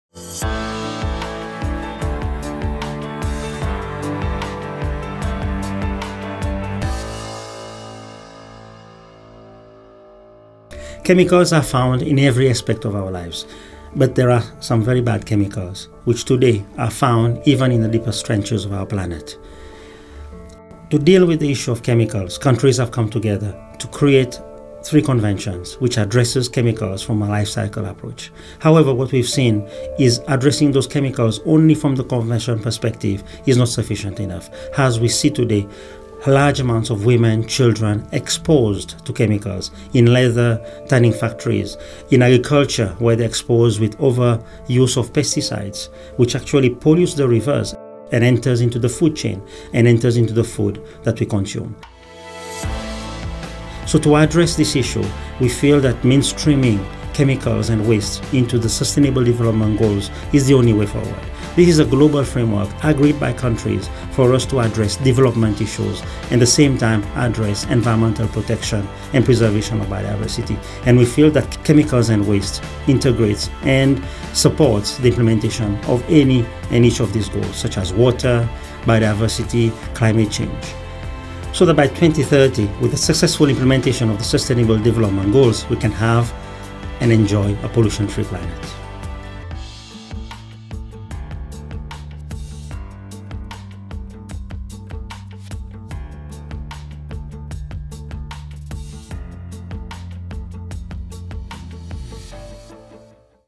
SDG Studio UN Geneva - SDG 12 - Rolph Payet
Rolph Payet, the Executive Secretary of the Basel, Rotterdam and Stockholm Conventions in the SDG Studio to talk about mainstreaming chemicals and waste into the SDGs to acheive SDG 12: Ensure sustainable consumption and production patterns